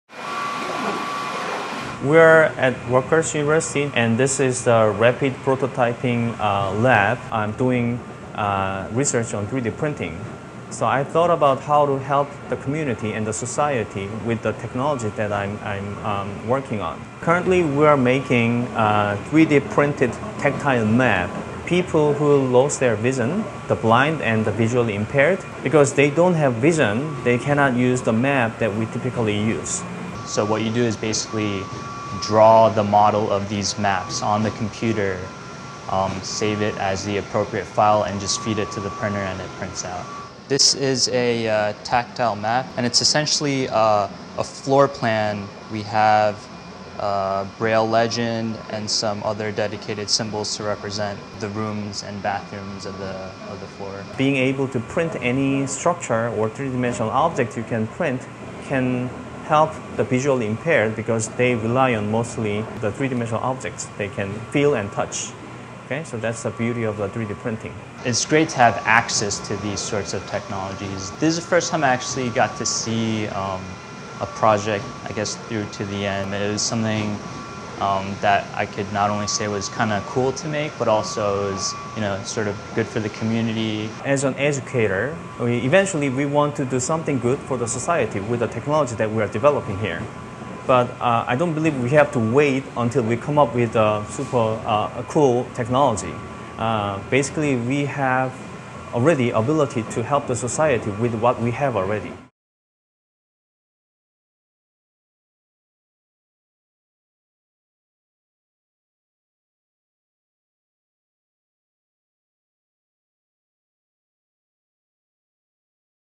3-D Printer Used To Help The Blind & Visually Impaired
A Rutgers professor and student are using the latest in technology to help the visually impaired. Using a 3D printer, they have created tactile maps that can be used to navigate buildings.